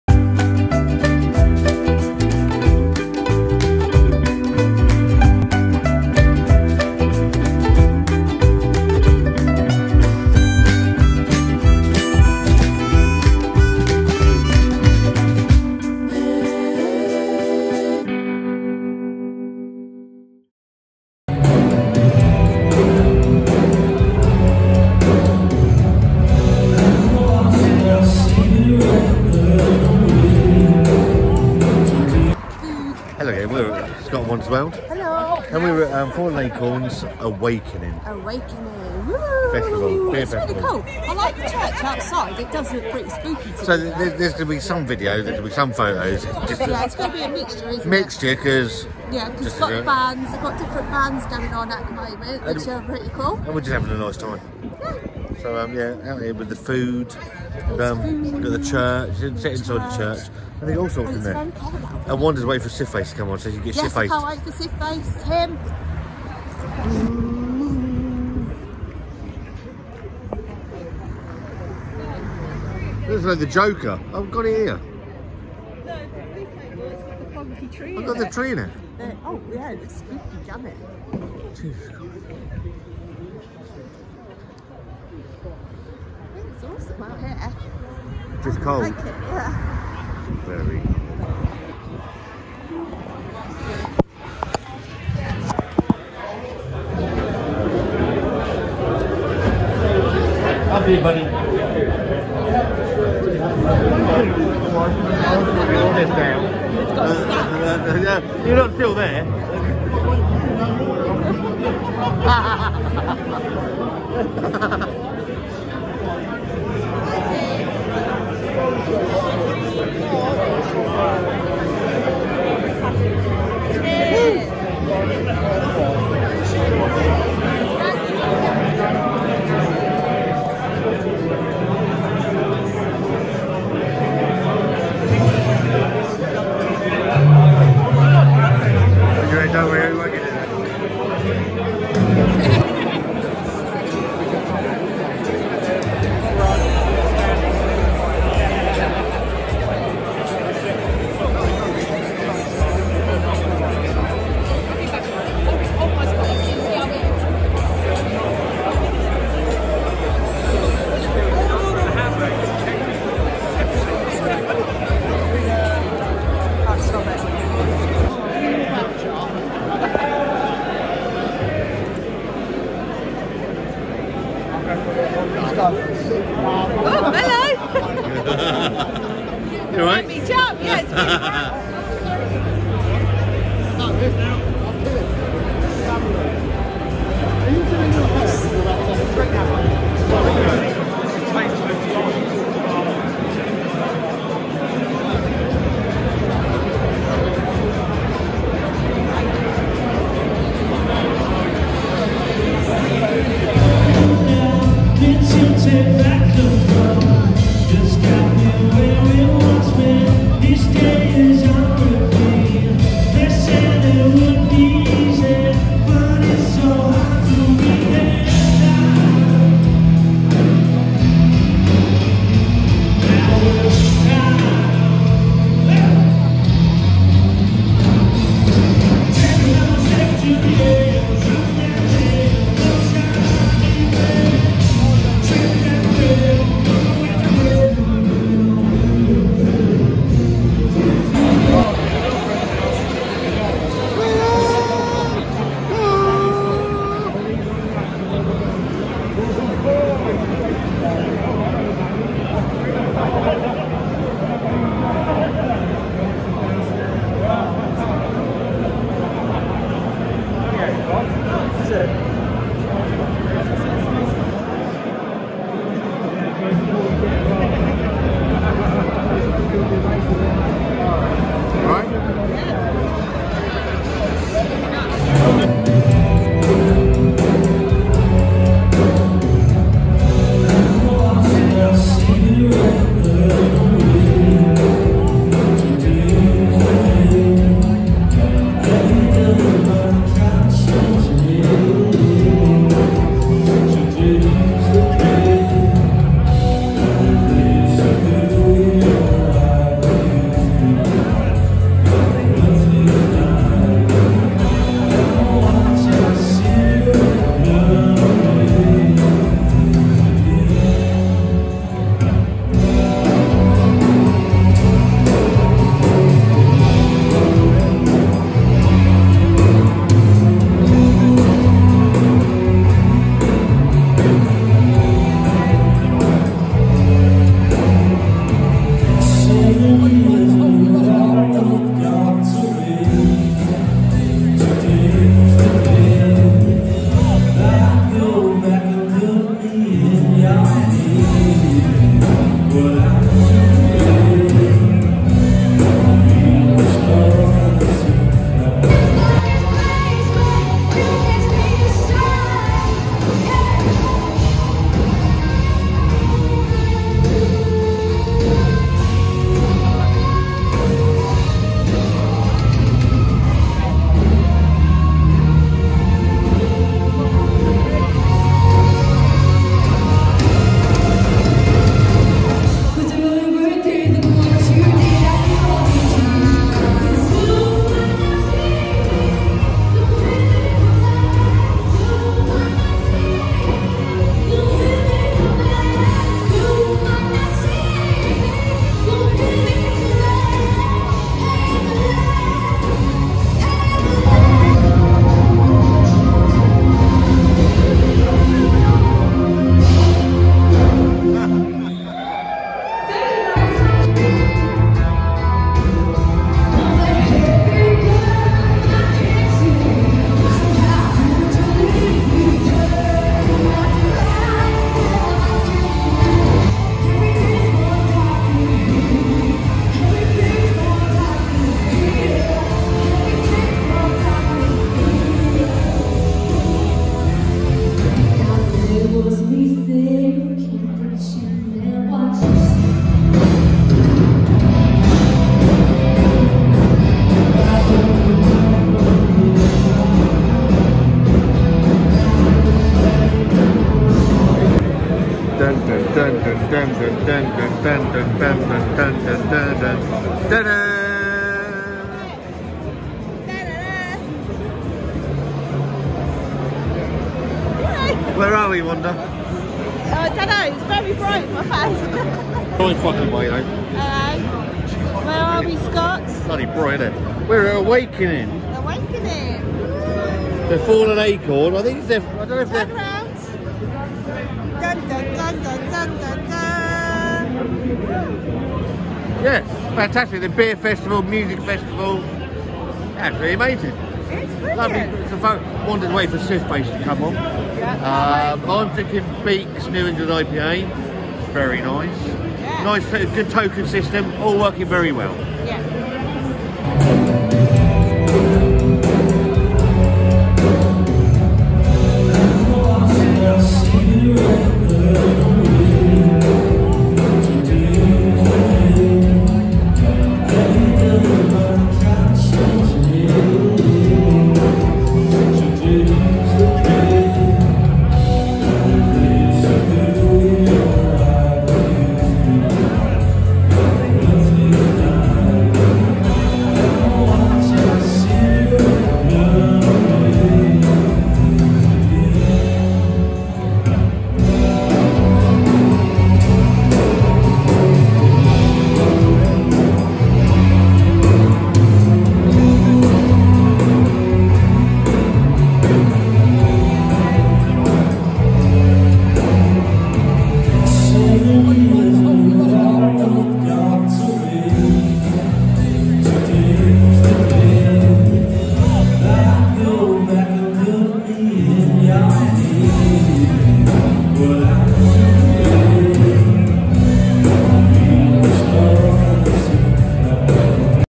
Awakening 2022 - Beer & Music Festival - Fallen Acorn Brewery
Awakening 2022 Beer & Music Festival by Fallen Acorn Brewery in Gosport Hants.